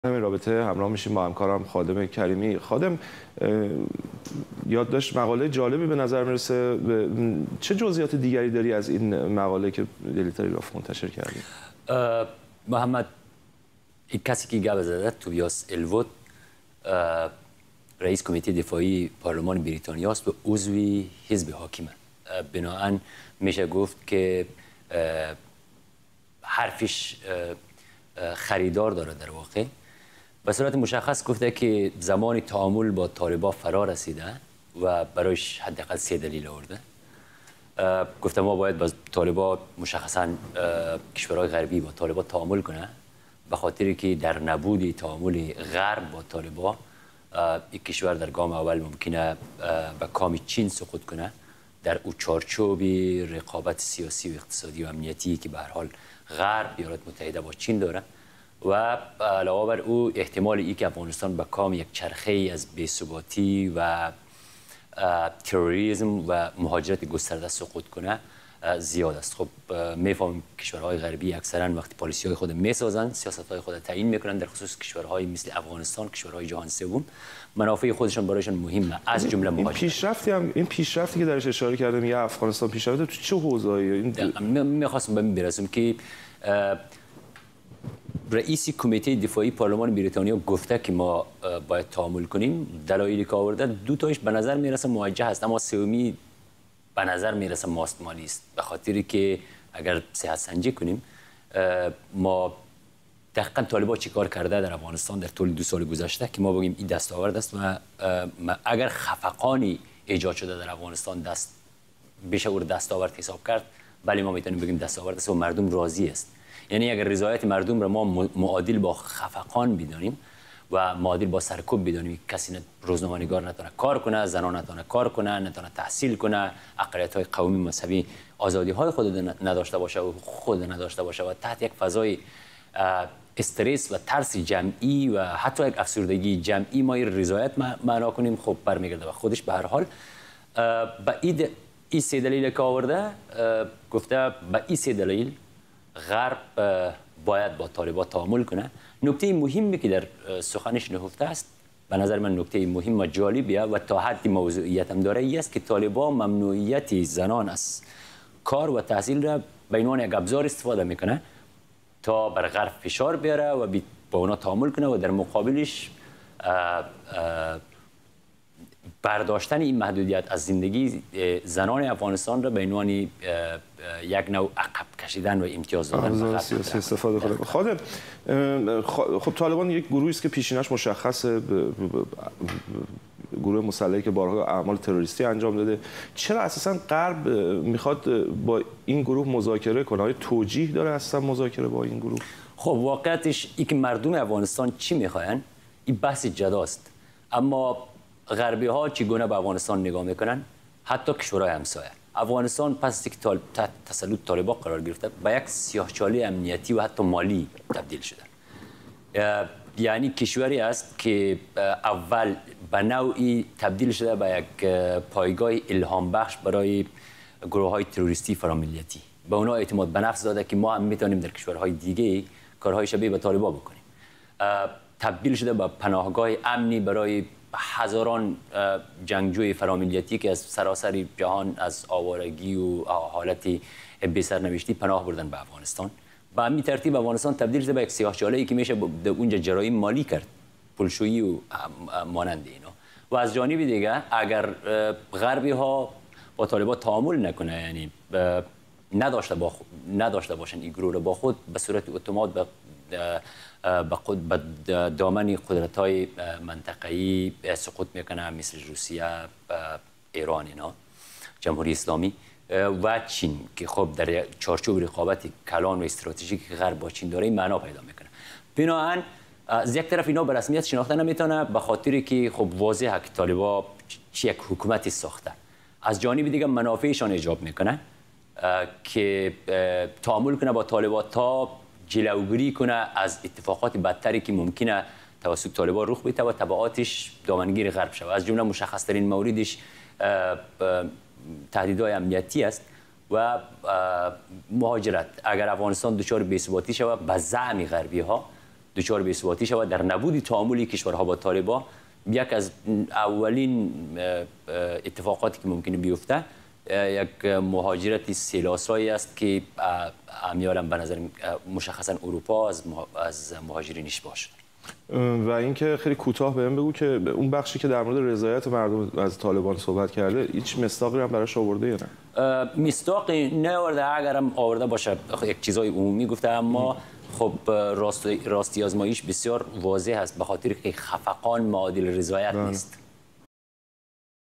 هشدار در مورد احتمال سقوط طالبان به کام چین
توبیاس الوود، رییس کمیته دفاعی پارلمان بریتانیا: غرب باید با طالبان تعامل کند

گفت‌وگو با خادم کریمی، ایران اینترنشنال